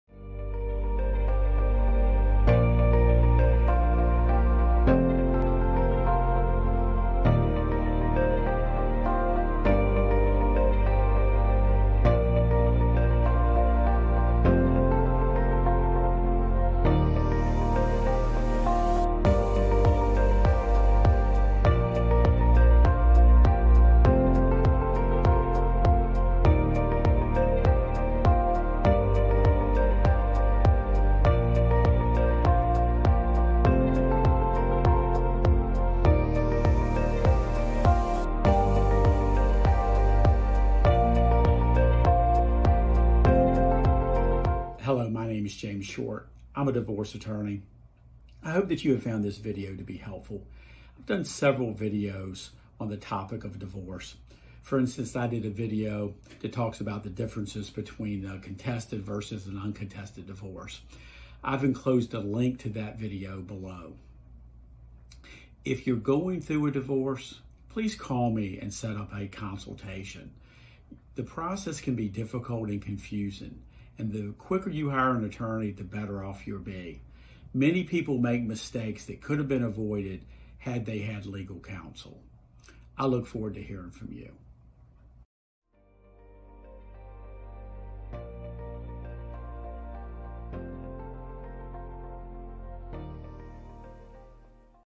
Divorce - What if I want an divorce, but my spouse does not?
0:00 Introduction
0:12 While it is much easier, faster, and cheaper to get divorced in Virginia if you and your spouse agree on the terms and cooperate with each other, you can still have a divorce finalized by the court without your spouse’s cooperation.
0:23 You can still get a divorce without a Separation Agreement.
0:35 Don't attempt your divorce without an attorney.
0:45 Video from James Short.
1:25 Contact information for James E. Short, PLC.

Here is another video that may be helpful regarding the difference between contested and uncontested divorces